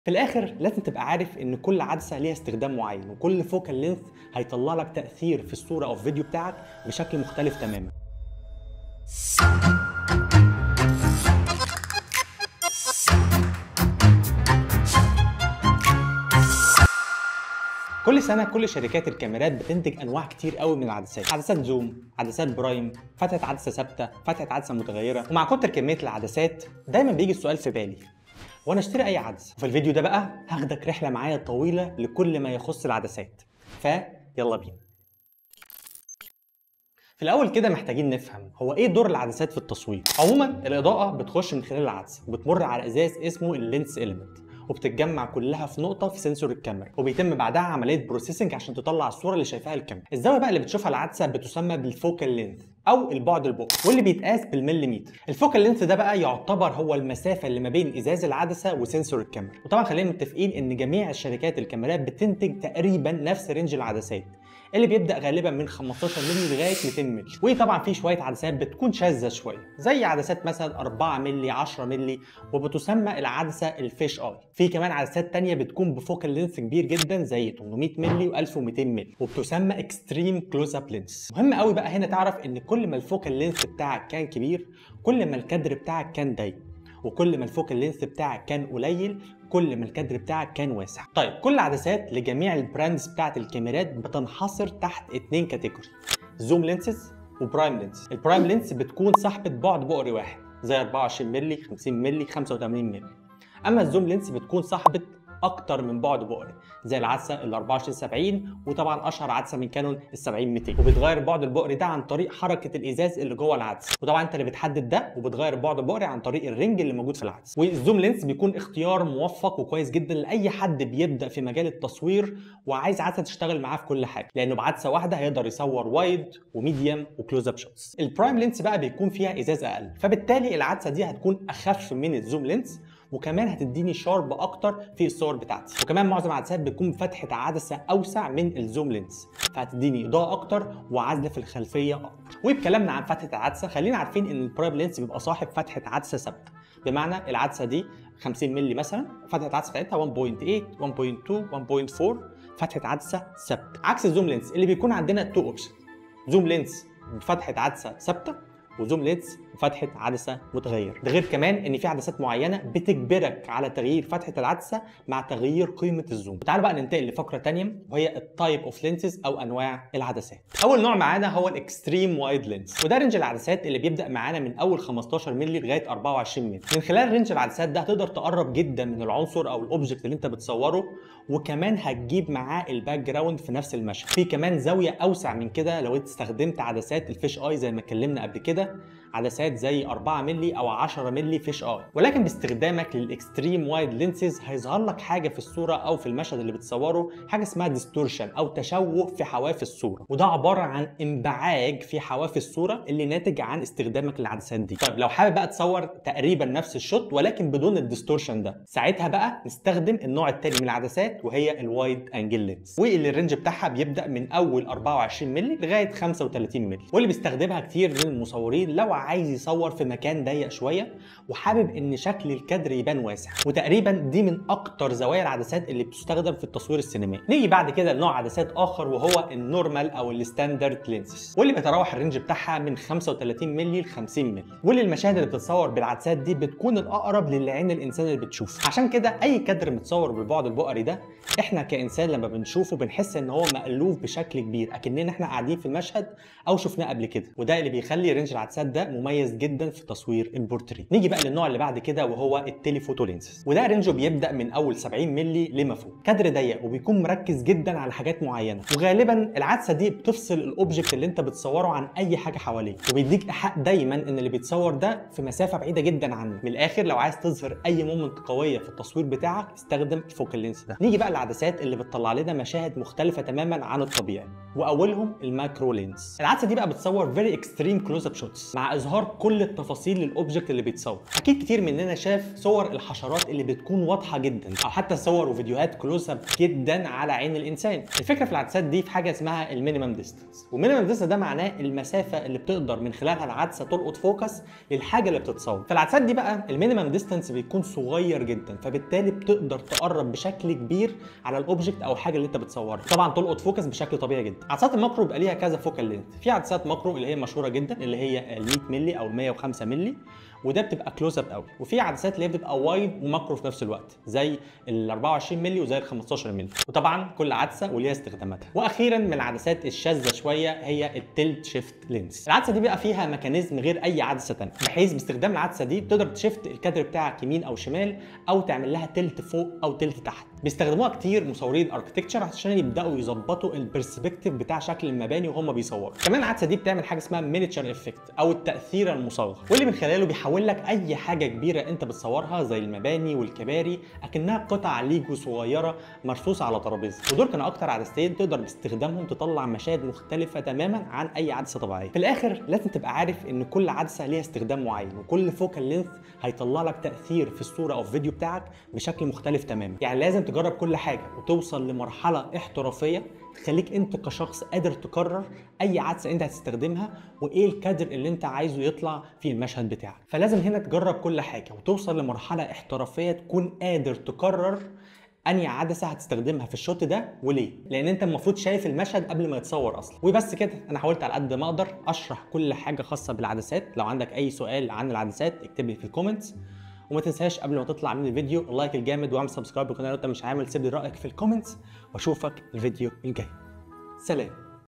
دليلك السريع لاختيار العدسة المناسبة للتصوير في 2024
دليلك السريع لاختيار العدسة المناسبة في 2024
كورس باقة الصمورين ( لايت روم - فوتوشوب ) ✅  👇
وسائل التواصل الإجتماعي 👇✅
متنساش تتفرج علي 👇👇
9 أسرار فى الفوتوشوب هيفرقوا فى شغلك 💯 - Photoshop Tips